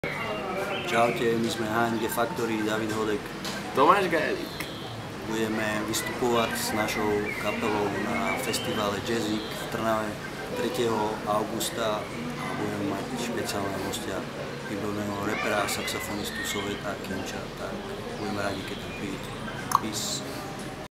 HG factory pozvanka na Jazzyk 2013
H&G Factory feat. Soweto Kinch - 3. augusta 2013 v trnavskom amfiteatri na festivale Trnavsky jazzyk